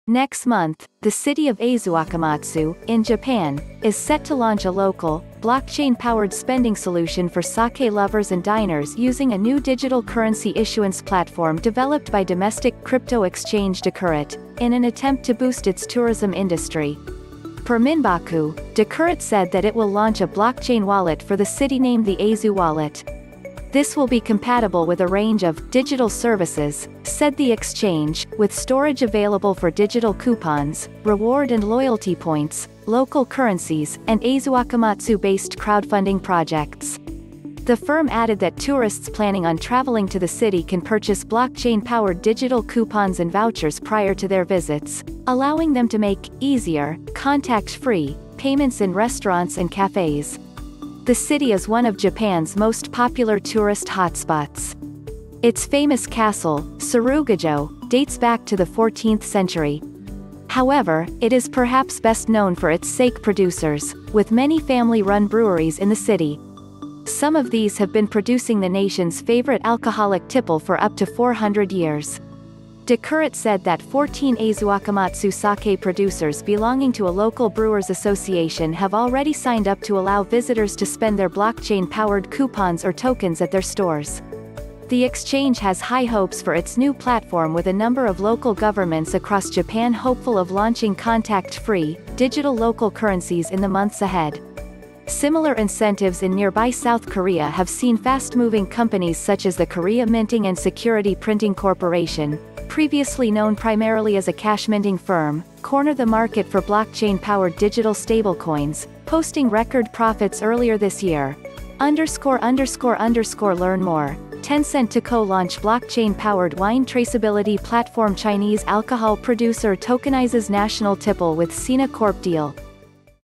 Booze loving Tourists To Buy Sake with Blockchain powered Coupons
Nex month, the city of Aizuwakamatsu, in Japan, is set to launch a local, blockchain-powered spending solution for sake-lovers and diners using a new digital currency issuance platform developed by domestic crypto exchange Decurret – in an attempt to boost its tourism industry. Per Minbaku, Decurret said that it will launch a blockchain wallet for the city named the Aizu Wallet. This will be compatible with a range of “digital services,” said the exchange, with storage available for digital coupons, reward and loyalty points, “local currencies” and Aizuwakamatsu-based crowdfunding projects. The firm added that tourists planning on traveling to the city can purchase blockchain-powered digital coupons and vouchers prior to their visits, allowing them to make “easier, contact-free” payments in restaurants and cafes. The city is one of Japan’s most popular tourist hotspots. Its famous castle, Tsurugajo, dates back to the 14th Century. However, it is perhaps best known for its sake producers – with many family-run breweries in the city. Some of these have been producing the nation’s favorite alcoholic tipple for up to 400 years. Decurret said that 14 Aizuwakamatsu sake producers belonging to a local brewers’ association have already signed up to allow visitors to spend their blockchain-powered coupons or tokens at their stores. The exchange has high hopes for its new platform with a number of local governments across Japan hopeful of launching contact-free, digital local currencies in the months ahead. Similar incentives in nearby South Korea have seen fast-moving companies such as the Korea Minting and Security Printing Corporation, previously known primarily as a cash-minting firm, corner the market for blockchain-powered digital stablecoins, posting record profits earlier this year.___Learn more:Tencent to Co-launch Blockchain-powered Wine Traceability PlatformChinese Alcohol Producer Tokenizes National Tipple with Sina Corp Deal